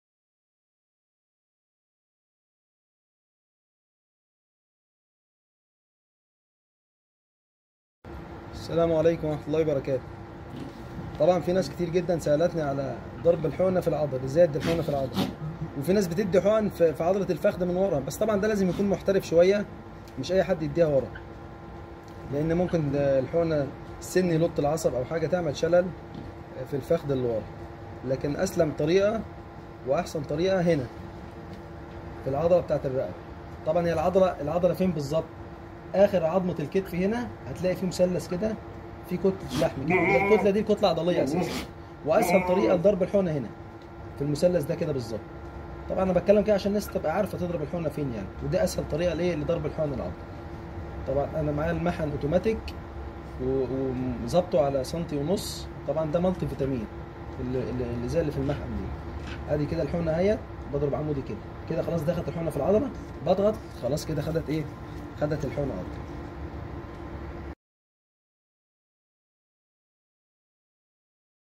الشيخ محمد حمود ابو عبدالملك كيفيه حقن الماعز
ماعز البور والقبرصي في مصر